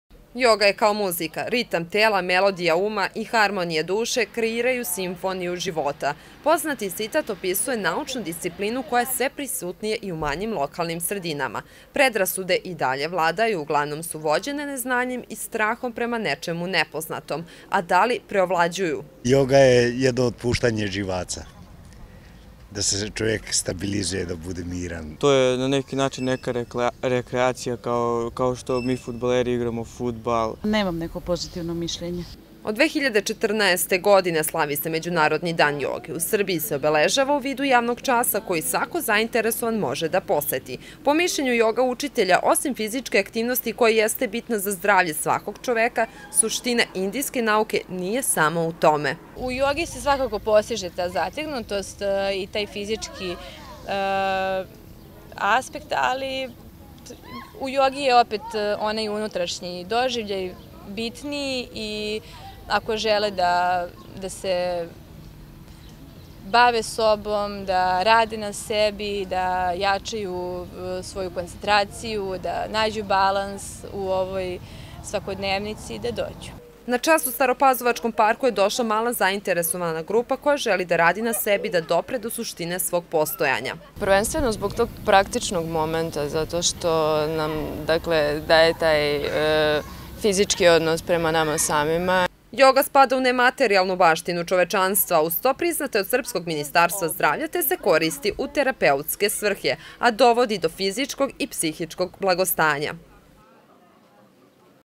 Medjunarodni dan joge
U parku u Staroj Pazovi danas je održan javni čas joge povodom Međunarodnog dana joge. Da li je zastupljena u našoj sredini i šta nam pruža ova naučna disciplina pogledajmo u prilogu koji sledi